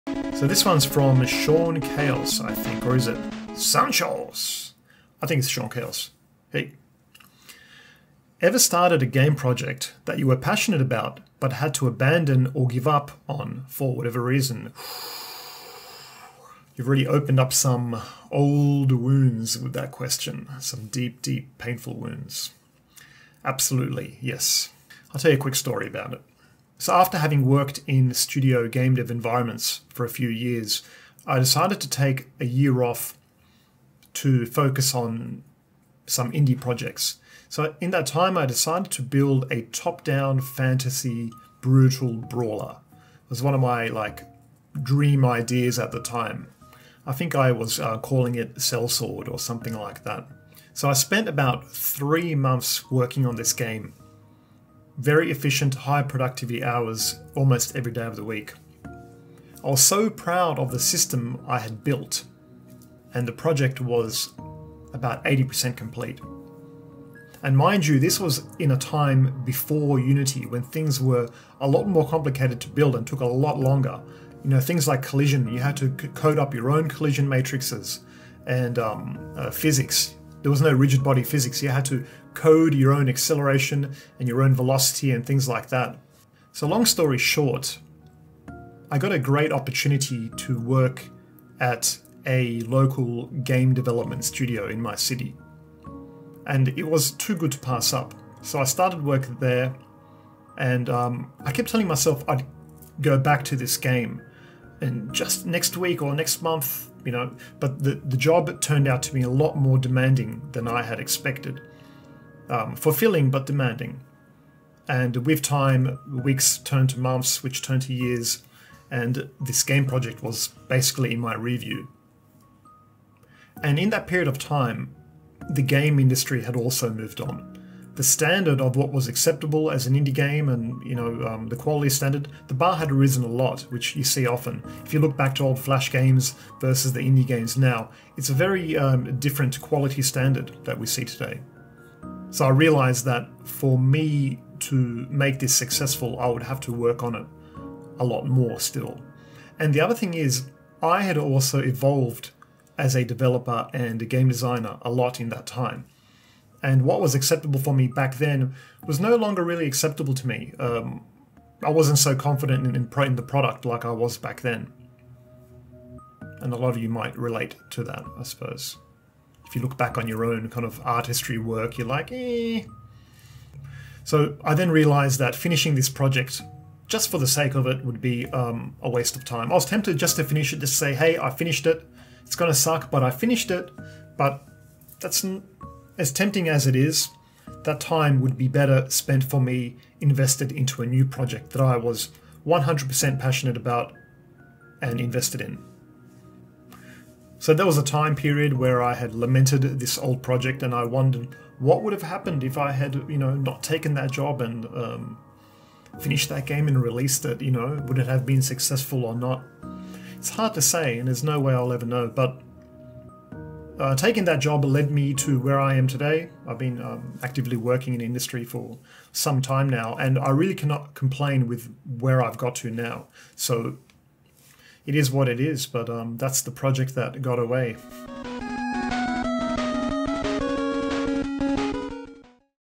The Game I had to let go after months of dev (Game Dev Q&A)
Thanks to all who have Send in the Game Dev Q&A. I will be releasing more over the next coming days!